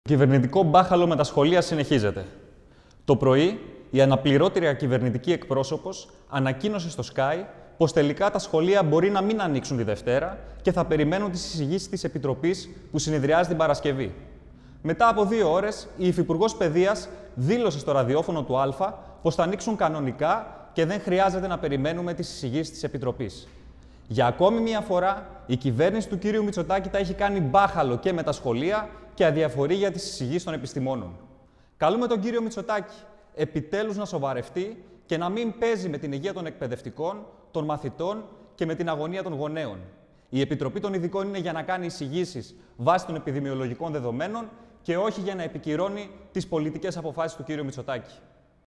Στ. Καλπάκης: Το κυβερνητικό μπάχαλο με τα σχολεία συνεχίζεται
Δήλωση του Αναπληρωτή Εκπροσώπου Τύπου του ΣΥΡΙΖΑ - Προοδευτική Συμμαχία